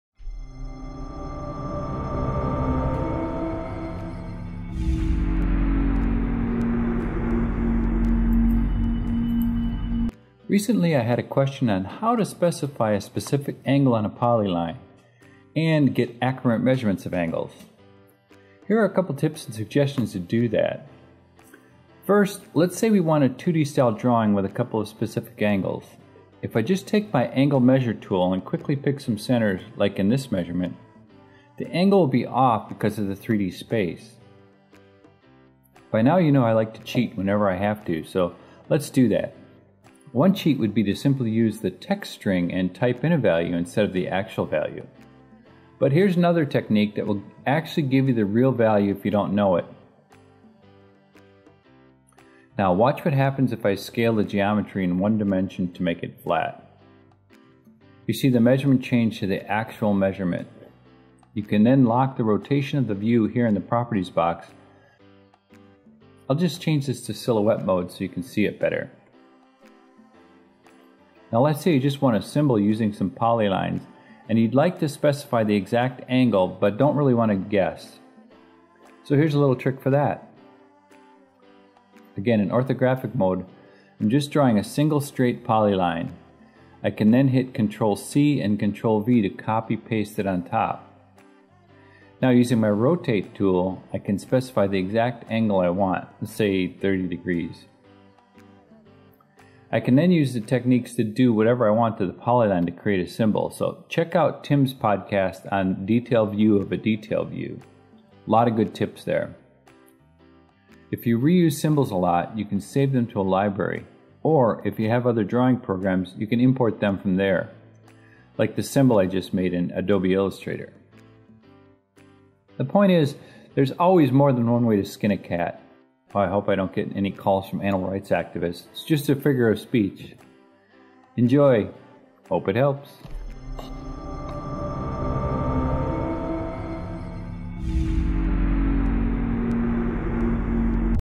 TodCast #21 Specify angles and correct measurements
Recently I got a question of how to specify a specific angle on a polyline and get accurate measurements of angles. Here are a couple of tips and suggestions to do that. Also, of you have not checked out Tim's Podcast of detail view of a detail view, check it out. It's a very powerful way to get just what you need.